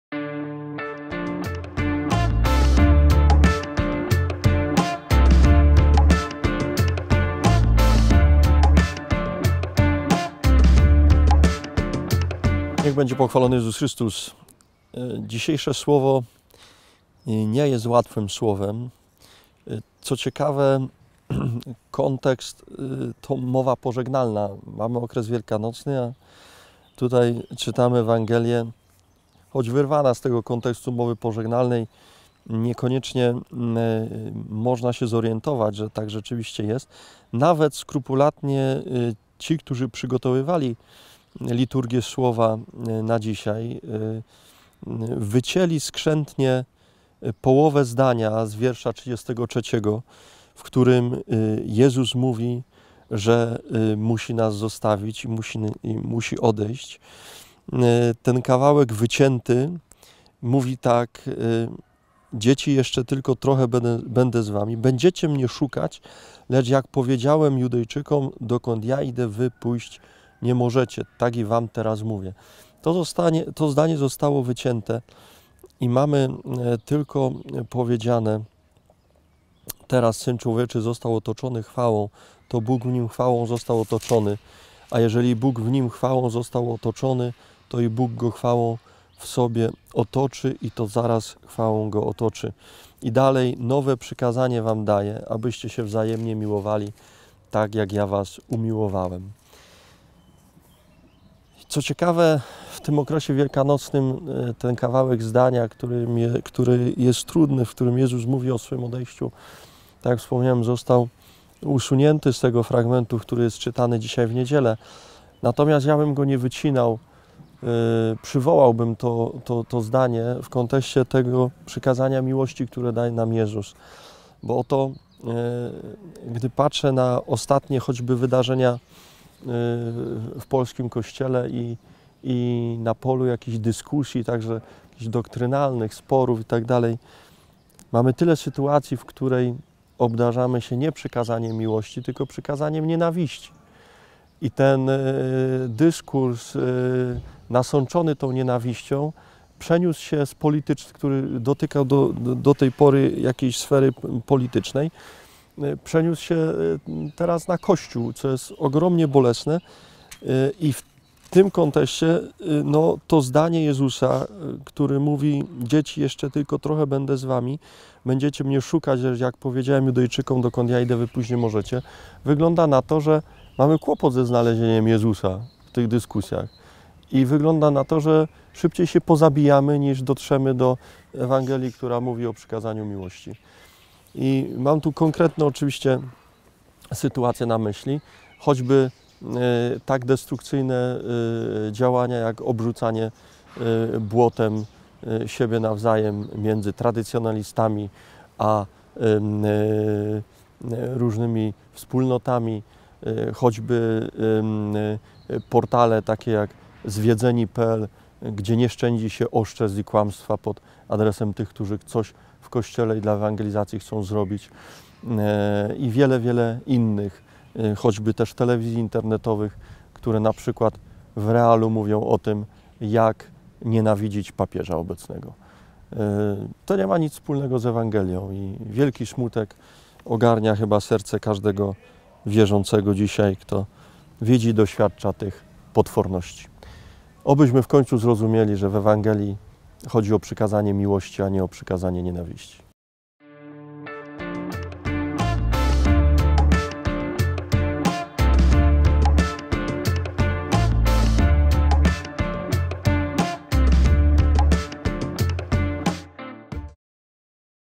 Na Fali Słowa - V Niedziela Wielkanocna - ks. Michał Olszewski SCJ
"Daję wam przykazanie nowe, abyście się wzajemnie miłowali, tak jak Ja was umiłowałem; żebyście i wy tak się miłowali wzajemnie. Po tym wszyscy poznają, żeście uczniami moimi, jeśli będziecie się wzajemnie miłowali”.